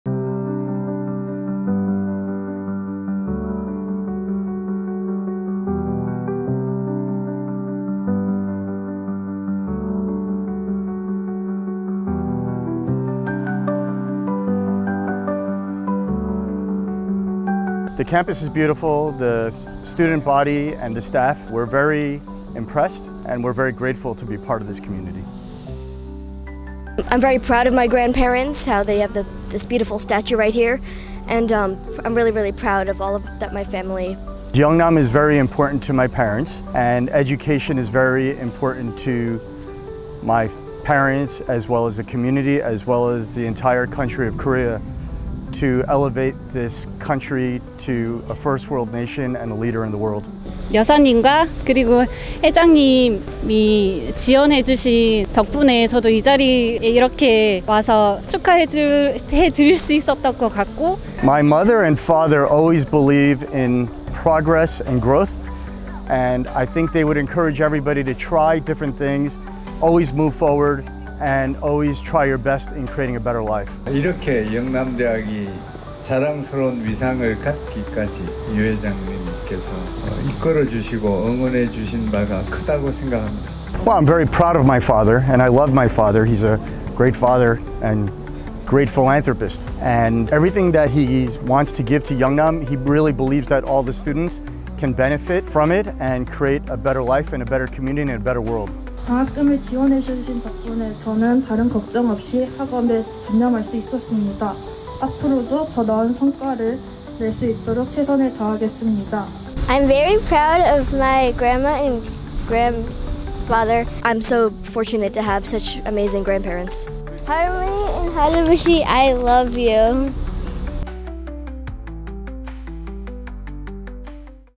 유득종 회장, 권순득 여사상 제막식
영남대학교에 많은 기부를 하신 유득종 회장님과 권순득 여사님을 위한 흉상이 중앙도서관 앞에 제작되었습니다.
이를 기념하기 위해 진행된 제막식 행사의 모습을 직접 담아보았습니다.

영상제작 : 영남대학교 홍보팀 x 이효림(사회학과 21) · 최정인(미디어커뮤니케이션 21)